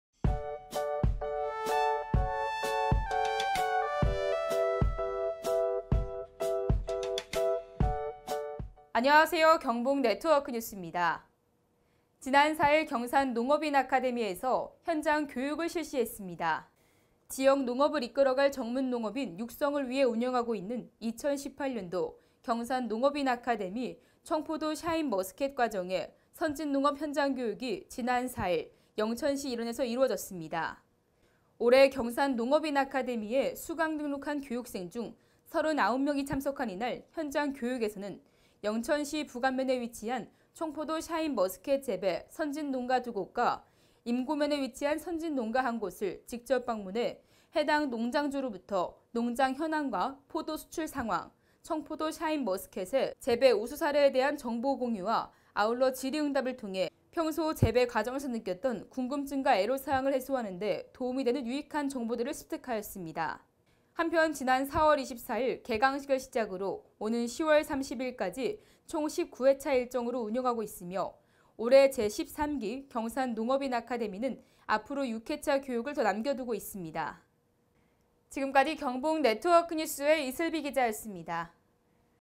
경산시청 최영조시장 경산농업인아카데미 청포도 샤인머스켓 현장 교육 경북포털 국민의소리TV 이슬비기자
지난 4일 경산농업인아카데미 현장 교육 실시

지역농업을 이끌어갈 전문농업인 육성을 위해 운영하고 있는 2018년도 경산농업인아카데미 청포도 샤인머스켓 과정의 선진농업 현장교육이 지난 4일 영천시 일원에서 이뤄졌다.

올해 경산농업인아카데미에 수강등록한 교육생 중 39명이 참석한 이날 현장 교육에서는 영천시 북안면에 위치한 청포도 샤인머스켓 재배 선진농가 2곳과 임고면에 위치한 선진농가 1곳을 직접 방문해 해당 농장주로부터 농장현황과 포도 수출상황, 청포도 샤인머스켓의 재배 우수사례에 대한 정보공유와 아울러 질의응답을 통해 평소 재배과정에서 느꼈던 궁금증과 애로사항을 해소하는데 도움이 되는 유익한 정보들을 습득하였다.

한편, 지난 4월 24일 개강식을 시작으로 오는 10월 30일까지 총 19회차 일정으로 운영하고 있는 올해 제13기 경산농업인아카데미는 앞으로 6회차 교육을 더 남겨두고 있다.